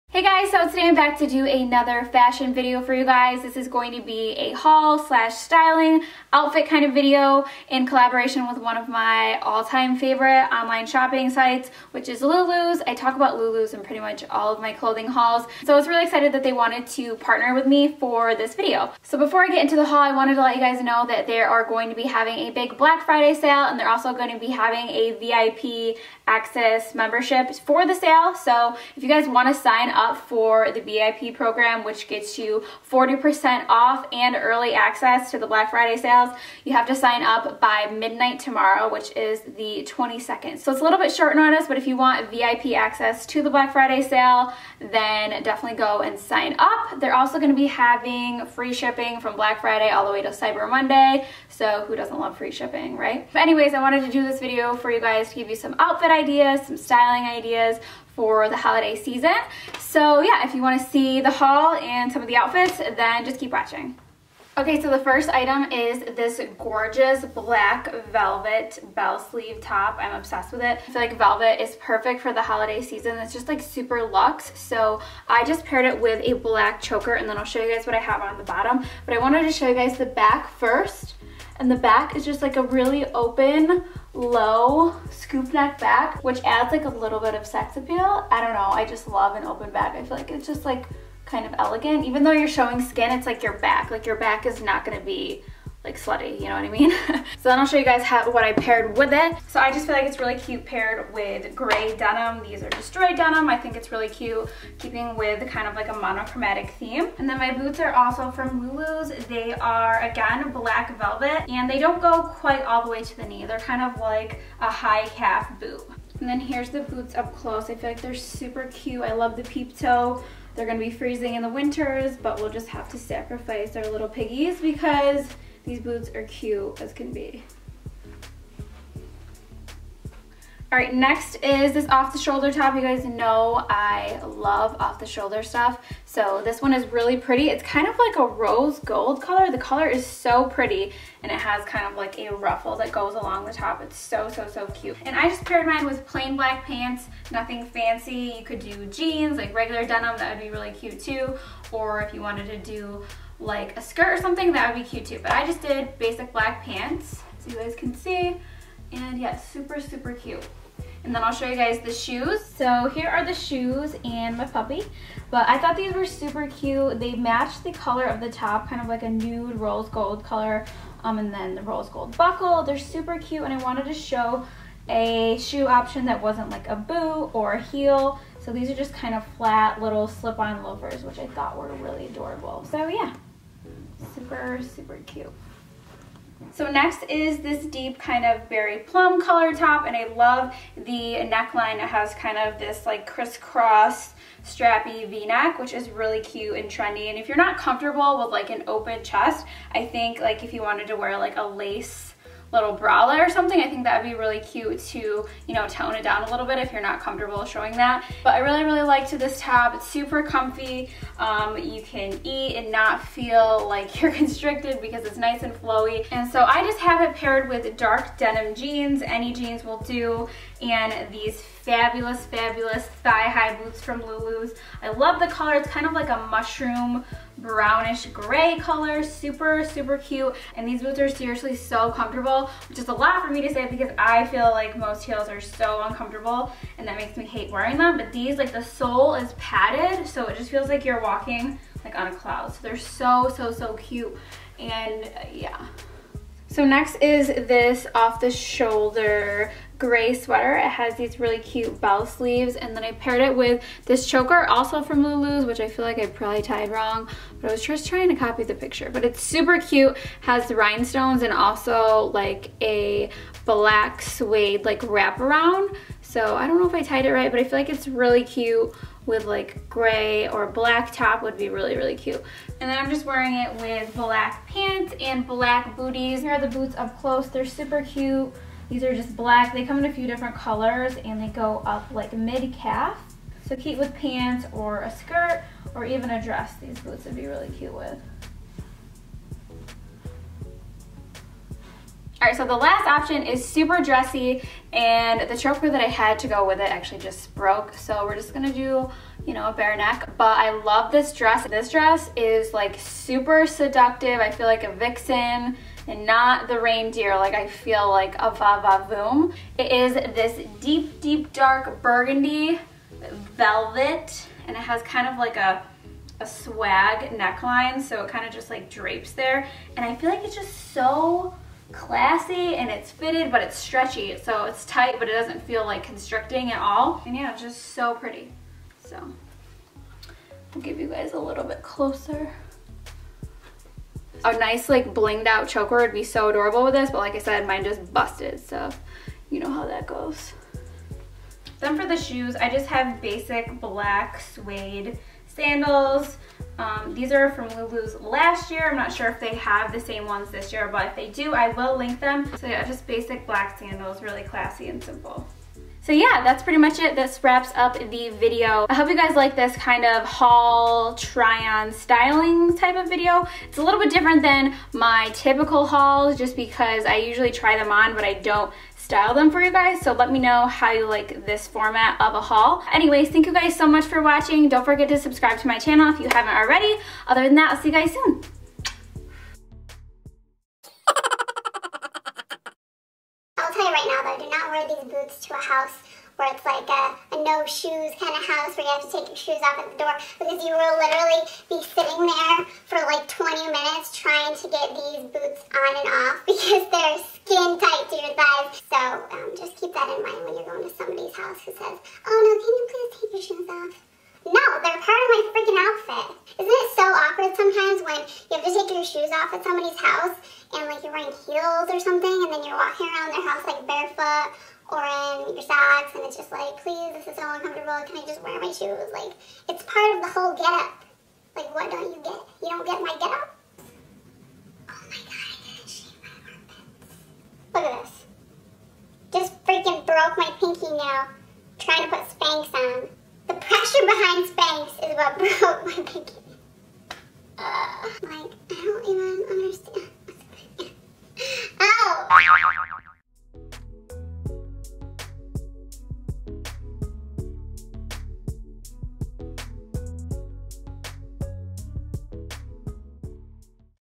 Holiday Fashion Haul + Outfit Ideas | Stephanie Ledda
Another clothing haul for fall/winter clothes & some holiday outfit ideas!!

HAVE YOU SEEN MY RECENT VIDEOS?

[make sure to use zeros!]

use XOXO for $$ off